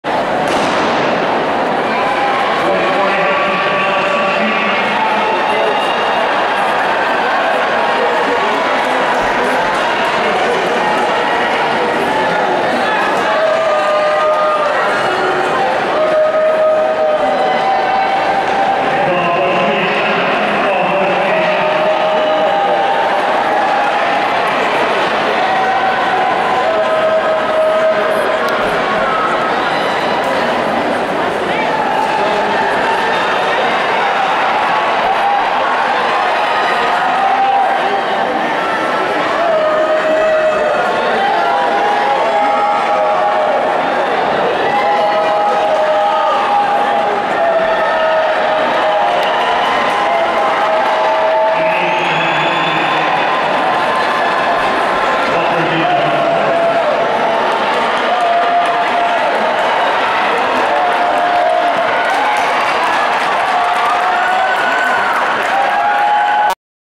Nate Gainey - 500m State Champ
Nate Gainey wins 500m - 2013 State Champion!  (Time = 1:06.66s)